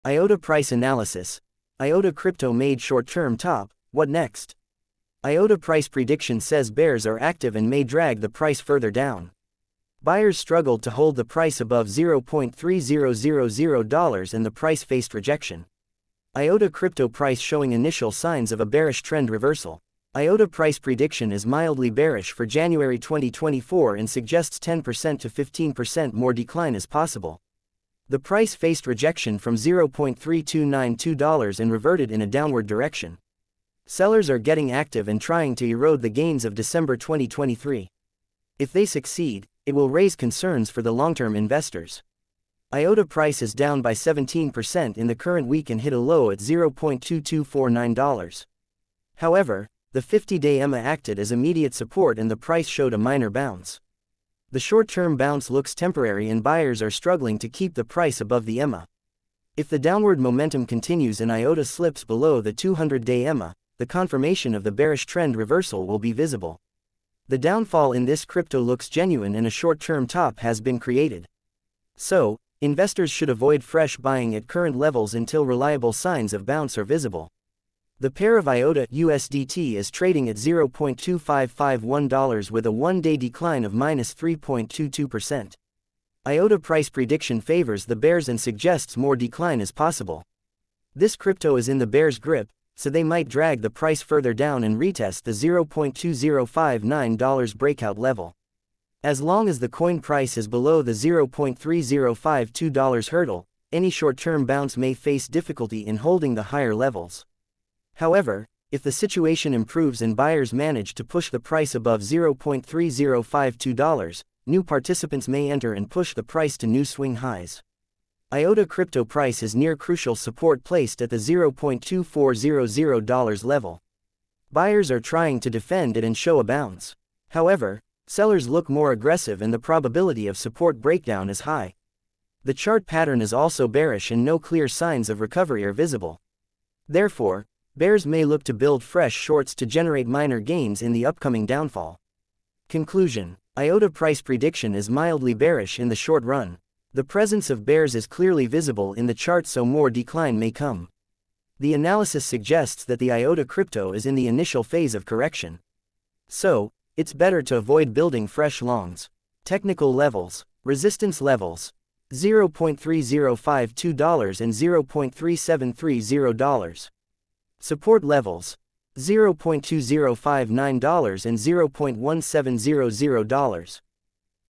IOTA Coin Price Prediction 2024 / IOTA Coin News Today / IOTA Coin Technical Analysis
Hello everyone. in this video, we shared IOTA coin analysis and IOTA price prediction with you.